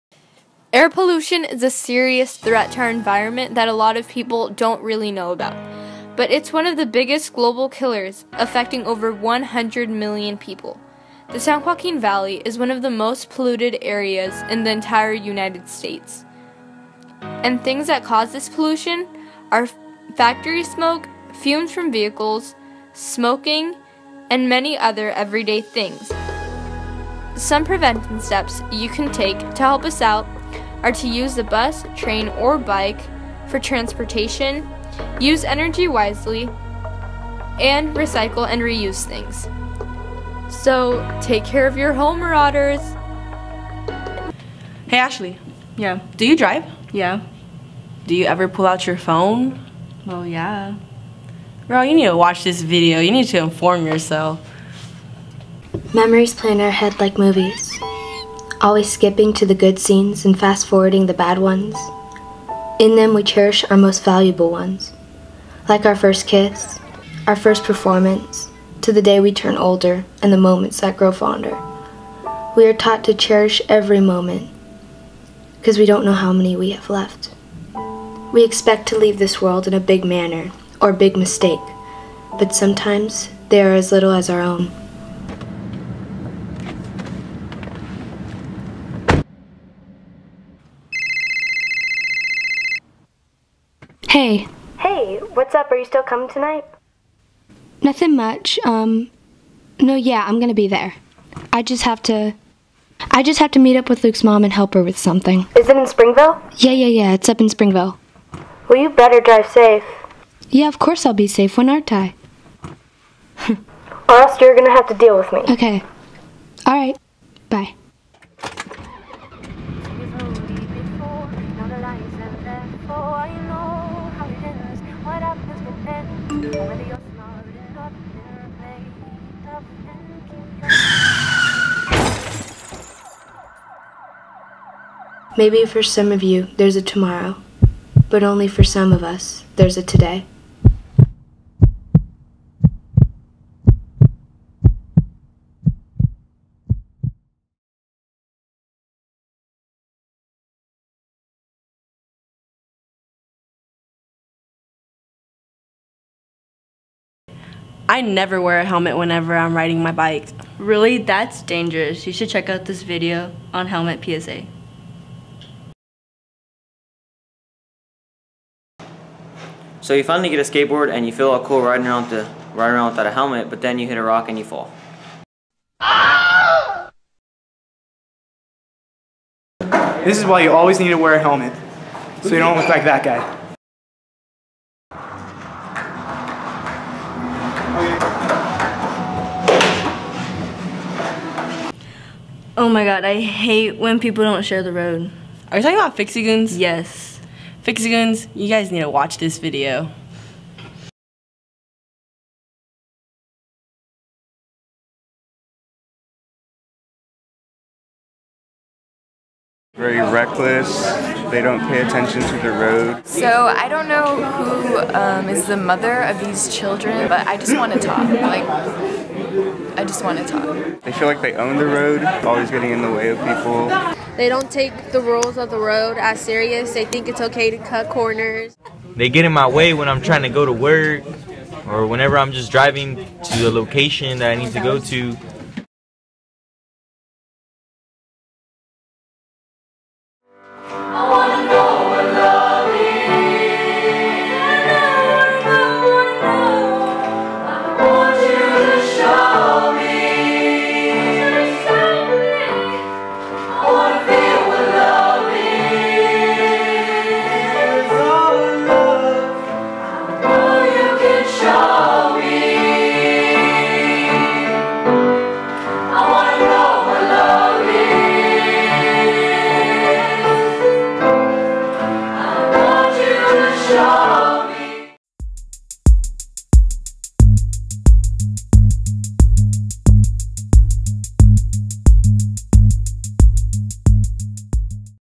MTV Broadcast - Monache High School - Season 1 - Episode 30 - 2/16/17
Previously known as Episode 25

Segment 1 - Air Pollution (Intro)
Segment 2 - Don't Text & Drive PSA
Segment 3 - Helmet PSA
Segment 4 - Fixie Goons
Segment 5 - Vote for Choir